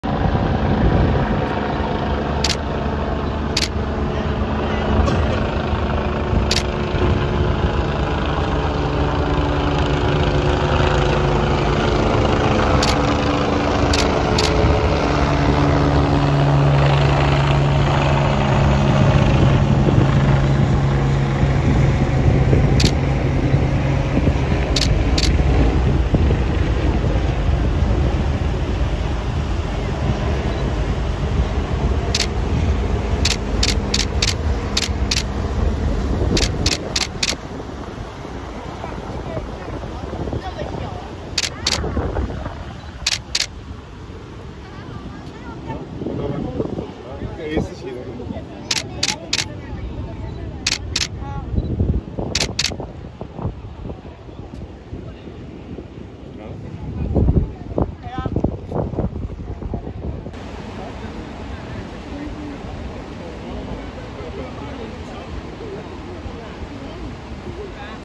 The famous Qiantang River tides!
The famous Qiantang River tides! The tides reach their peak on the 18th day of the eighth lunar month, which is today this year!